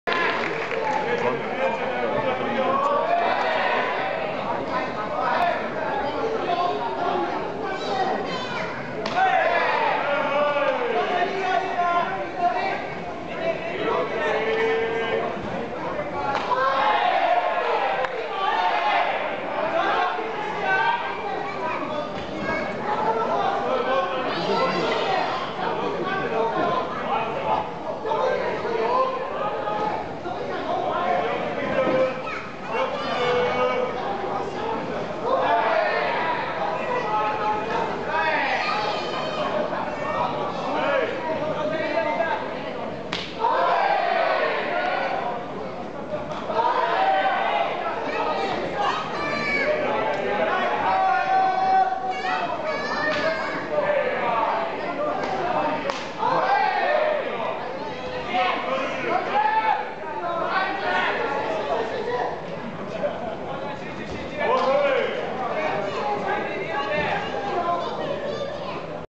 Togo vs. Yuki Takewaka
Magnum24 kickboxing fight, at Korakuen Stadium, Tokyo Dome